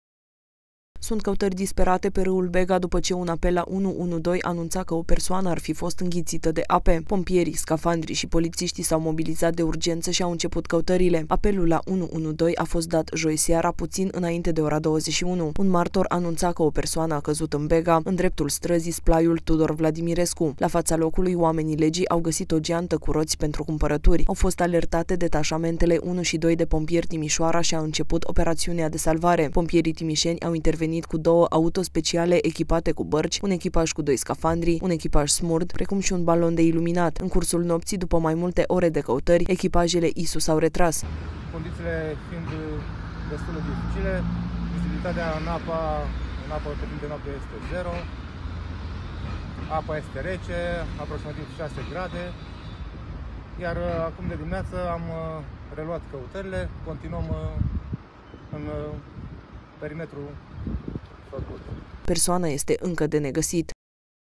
Persoană dispărută în Bega
Sunt căutări disperate pe râul Bega după ce un apel la 112 anunța că o persoană ar fi fost înghițită de ape. Pompierii, scafandrii și polițiștii s-au mobilizat de urgență și au început căutările. Apelul la 112 a fost dat joi seara, puțin înainte de ora 21. Un martor anunța că o persoană a căzut în Bega, în dreptul străzii Splaiul Tudor Vladimirescu. La fața locului, oamenii legii au găsit o geantă cu roți pentru cumpărături.